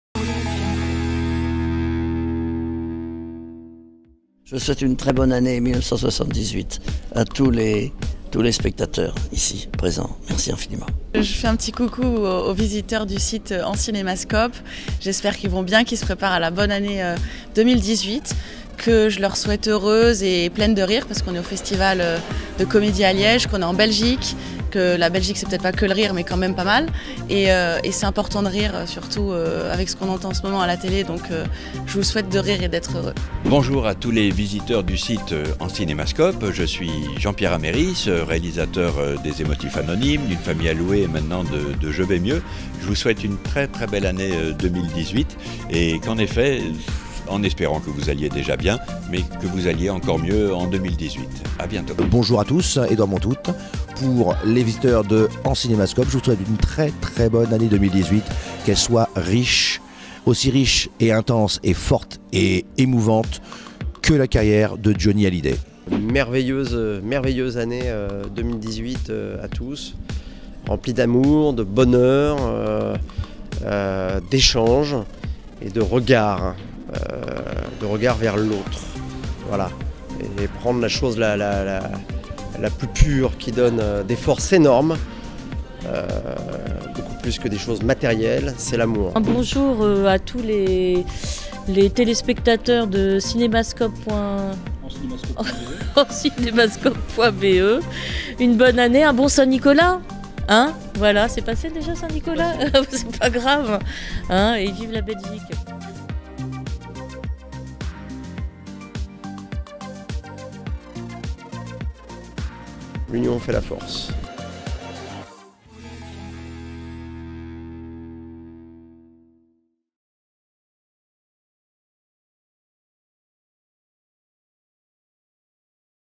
Bonne année 2018... en cinémascope !
Daniel Prévost, Anouchka Delon, Jean-Pierre Améris, Edouard Montoute, Frédéric Diefenthal et Catherine Benguigui vous souhaitent une bonne année 2018 avec tendresse et humour !